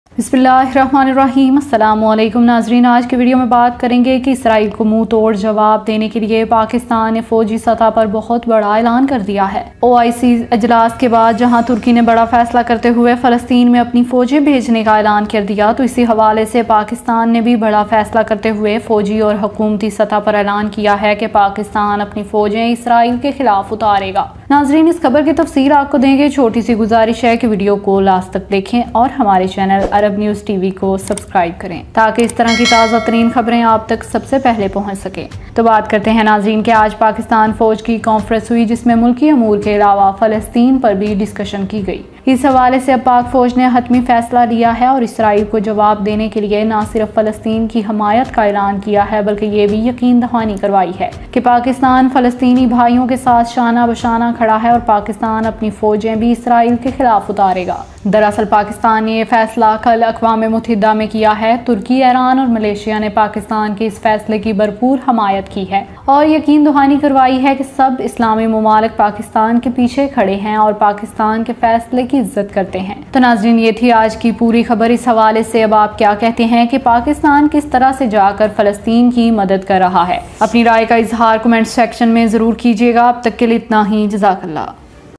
Pakistan Come On Front Against Israel On Palestine Issue | Israel-Palestine Conflict Update
Hello Friends In Today's Video I Will Tell You About Pakistan Come On Front Against Israel On Palestine Issue | Israel-Palestine Conflict Update | By Arab News TV In Hindi Urdu

|Saudi Arabia Riyal Exchange Rate Today| |19 May 2021| |Today Riyal Exchange Rate All Countries| |Arab News TV|